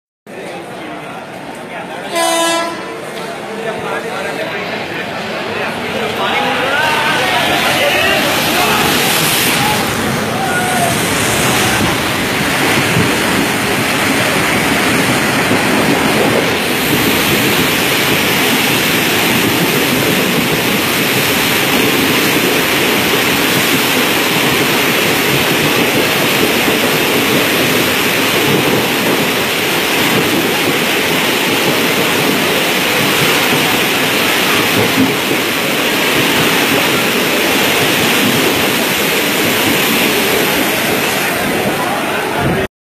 വണ്ടർലാ ഒന്നുമല്ല. Latest ride in kerala
എറണാകുളം സൗത്ത് റെയിൽവേ സ്റ്റേഷൻ
വീഗാലന്റിൽ കിട്ടുവോ ഇത്രേം നല്ല റൈഡ്

Watch